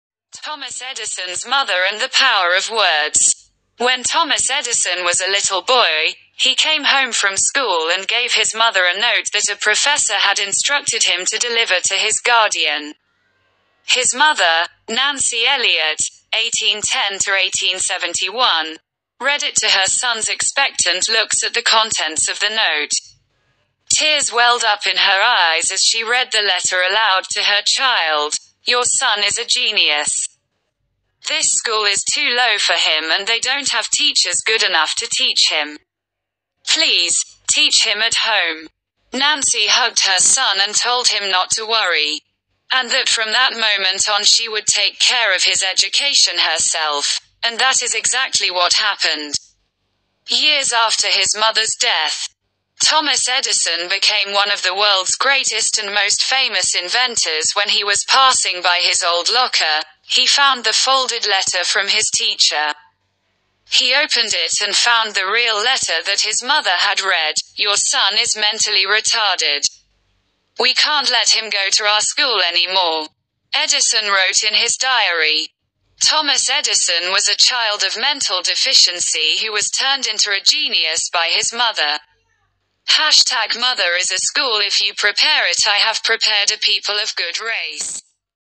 Thomas Edison and his mother and the power of words
When Thomas Edison was a little boy, he came home from school and gave his mother a note that a professor had instructed him to deliver to his guardian.
 His mother, Nancy Elliott (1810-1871), read it to her son's expectant looks at the contents of the note.

 Tears welled up in her eyes as she read the letter aloud to her child: "Your son is a genius. This school is too low for him and they don't have teachers good enough to teach him. Please, teach him at home."

 Nancy hugged her son and told him not to worry, and that from that moment on she would take care of his education herself, and that is exactly what happened.

 Years after his mother's death, Thomas Edison became one of the world's greatest and most famous inventors

 When he was passing by his old locker, he found the folded letter from his teacher.  He opened it and found the real letter that his mother had read

  “Your son is mentally retarded. We can't let him go to our school anymore.”

 Edison wrote in his diary: “Thomas Edison was a child of mental deficiency who was turned into a genius by his mother.”

Mother is a school if you prepare it
 I have prepared a people of good race